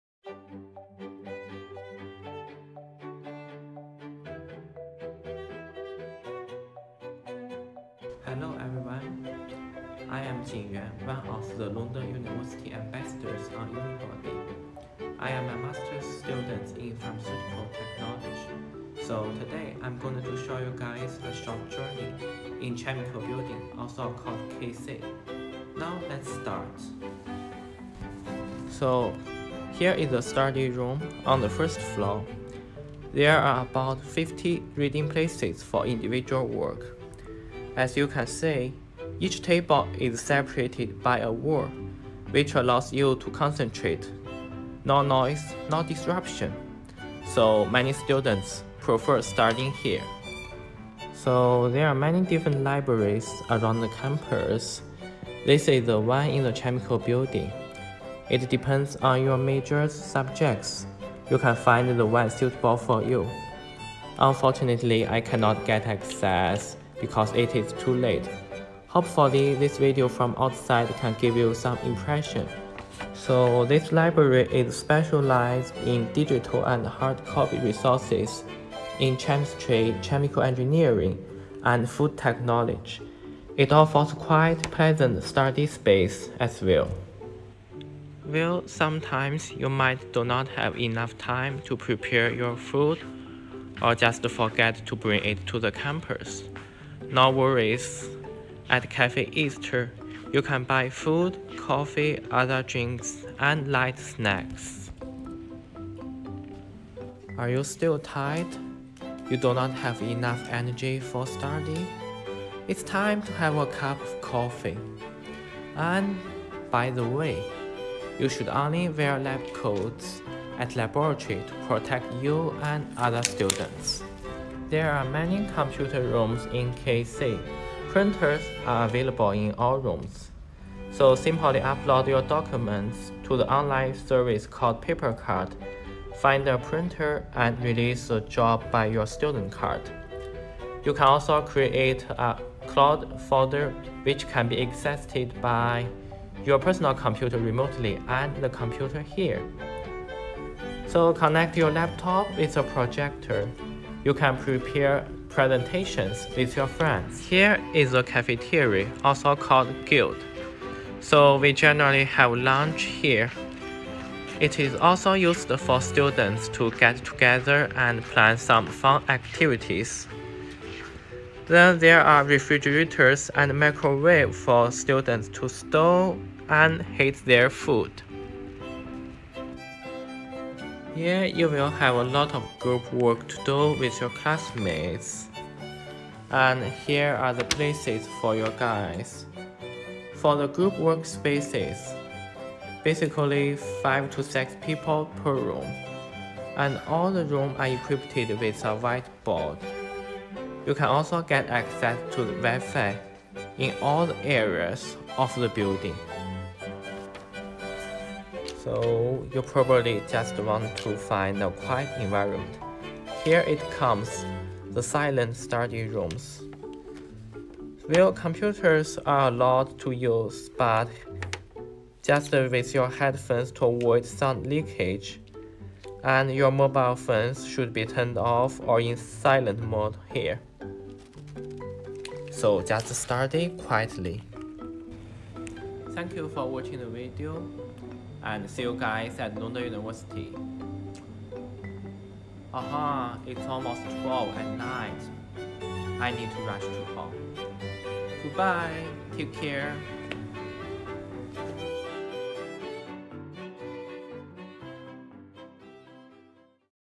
A Short Campus Tour in the Chemistry Building, LTH
Hello, I am a Lund University international student ambassador studying for the Master's programme in Pharmaceutical Technology.
Are you curious about how is the student life like on campus? In this Vlog, I will bring you on a short journey in the Chemical Building, LTH, with a short description of the study places, library, computer rooms and Café.
Let's Start!

To chat more with Jinyuan or over 80 other current students